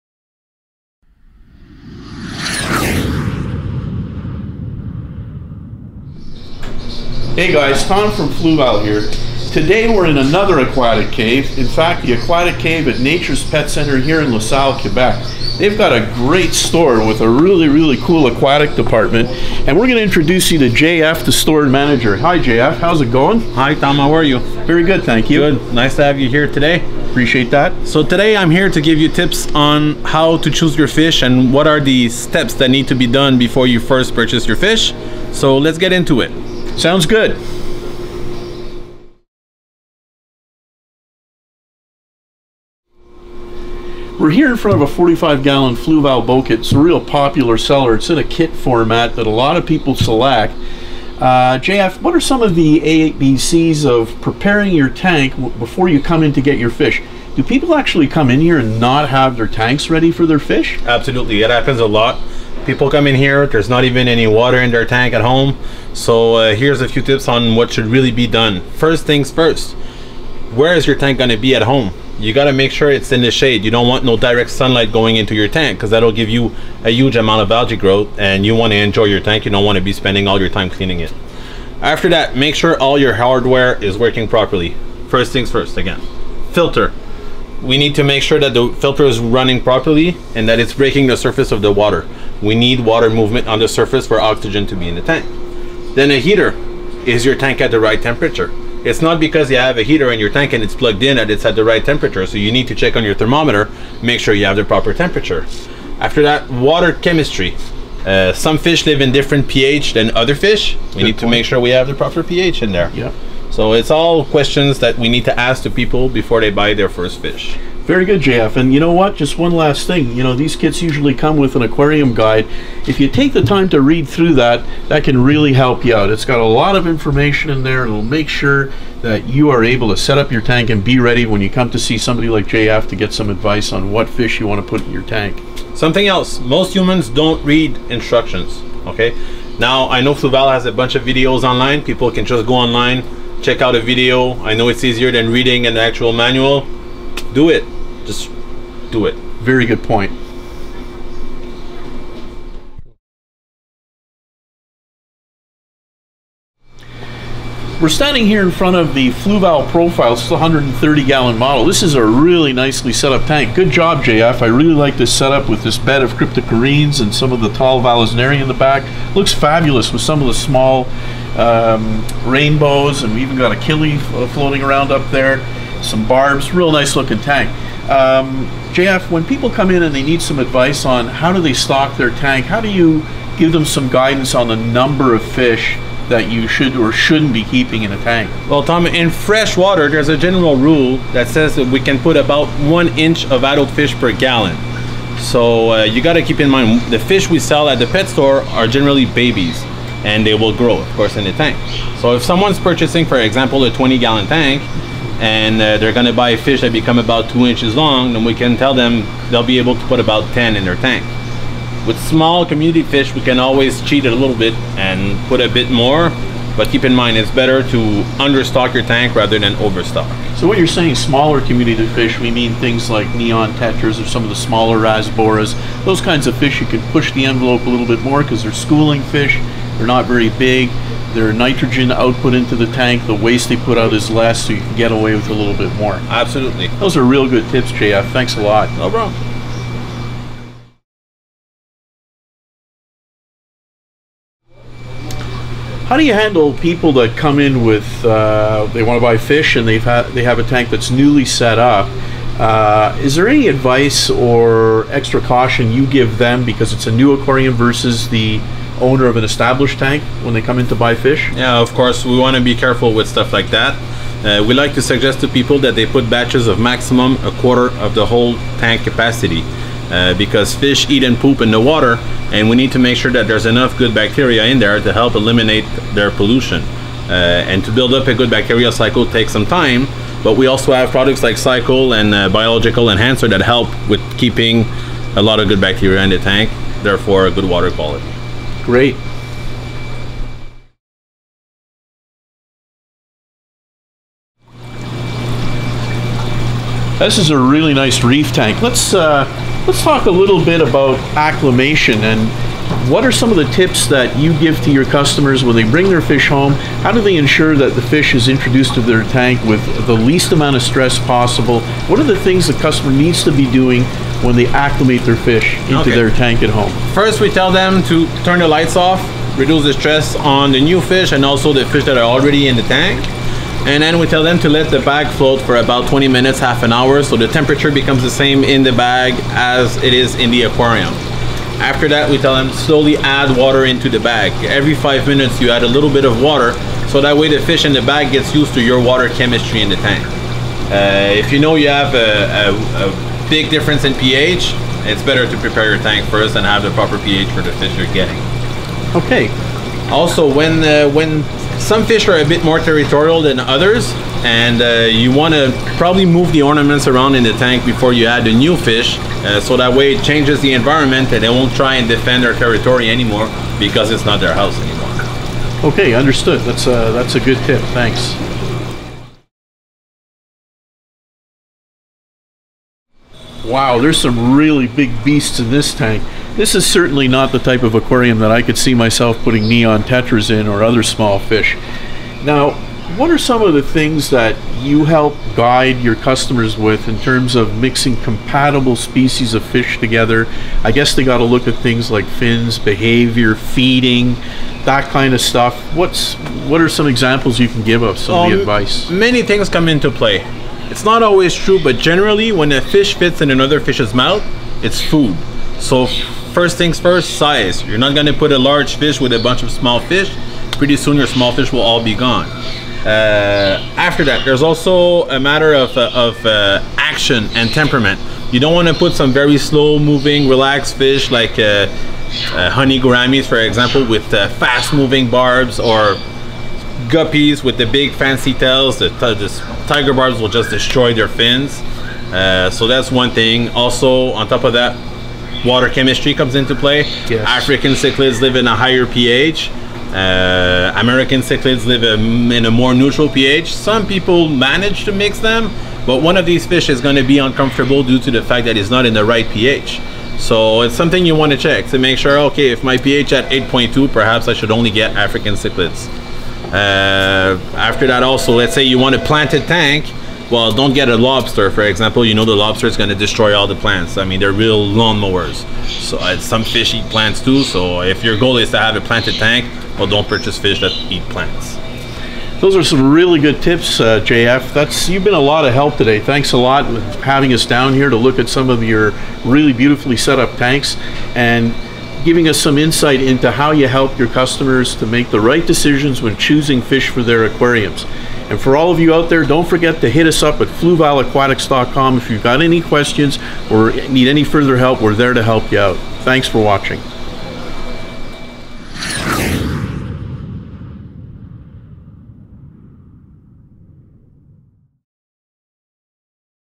Tips for Buying Aquarium Fish
Tom Sarac from Fluval meets up with J.F. Pujolle at Nature Pet Centre in Lasalle, Quebec to uncover the Top 5 tips for buying fish, including selecting the right species, proper aquarium set-up, understocking versus overstocking, and more. To view the Fluval Aquarium Care Guide, click on the following